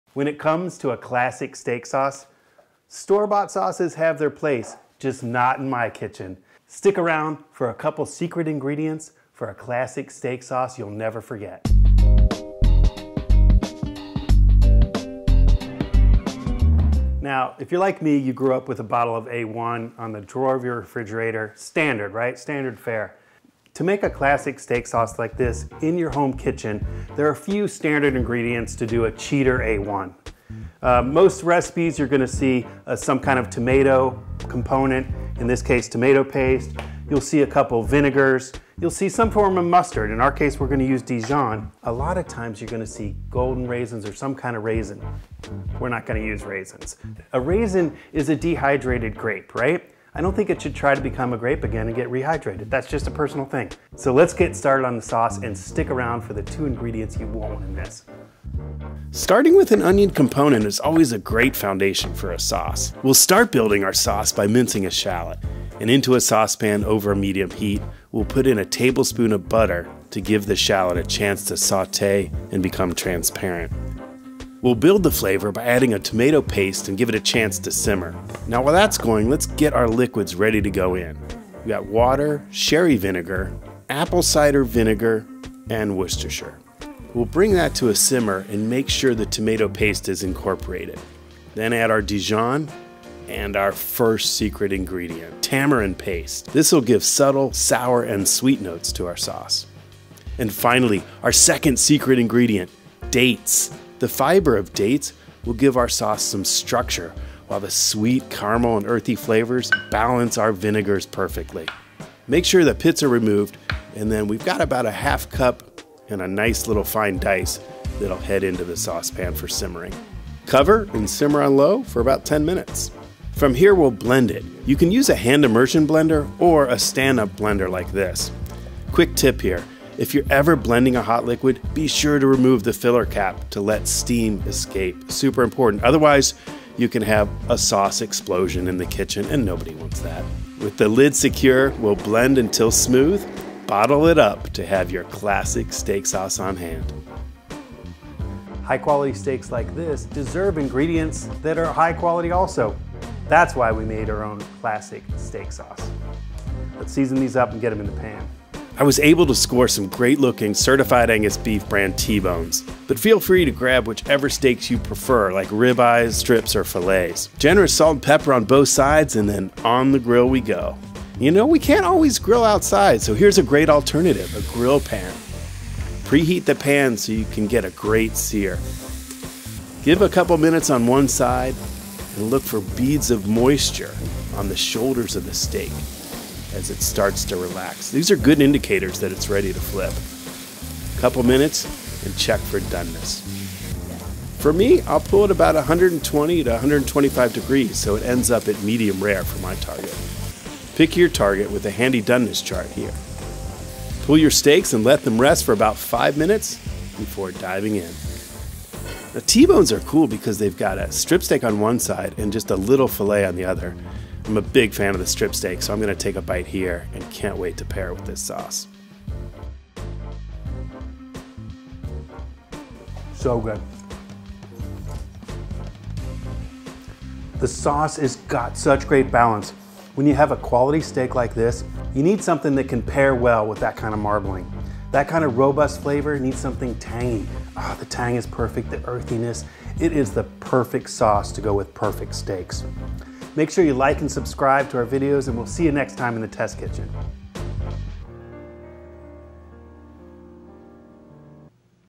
Better Than A.1. | How to Make a Classic Steak Sauce
Forget bottled steak sauce -- this classic homemade version is tasty and simple. And sure, T-bones and other steaks are delicious by themselves, but this rich, thick and flavorful sauce pairs perfectly with grilled steaks. It also adds a whole new level of "Wow!" to your favorite steak!

INGREDIENTS:
4 (16-ounce) Certified Angus Beef ® T-bone steaks
2 teaspoons salt
1 teaspoon pepper
1 shallot, diced (about 1/4 cup)
1 tablespoon butter
1 tablespoon tomato paste
1 cup water
1/3 cup apple cider vinegar
1/3 cup sherry vinegar
2 tablespoons Worcestershire sauce
2 teaspoons Dijon mustard
2 teaspoons tamarind paste
1/2 cup seeded and roughly chopped Medjool dates